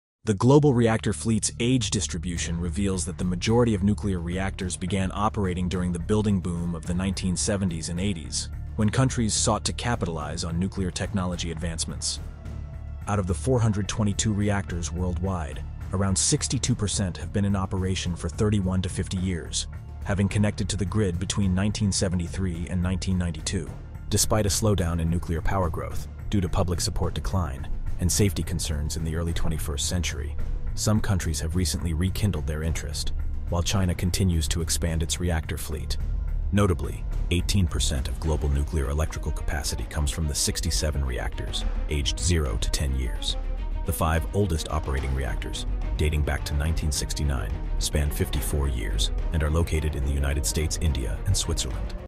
World’s Oldest Nuclear Reactors
The global reactor fleet's age distribution reveals that the majority of nuclear reactors began operating during the building boom of the 1970s and 80s, when countries sought to capitalize on nuclear technology advancements. Out of the 422 reactors worldwide, around 62% have been in operation for 31 to 50 years, having connected to the grid between 1973 and 1992. Despite a slowdown in nuclear power growth due to public support decline and safety concerns in the early 21st century, some countries have recently rekindled their interest, while China continues to expand its reactor fleet. Notably, 18% of global nuclear electrical capacity comes from the 67 reactors aged zero to 10 years. The five oldest operating reactors, dating back to 1969, span 54 years and are located in the United States, India, and Switzerland.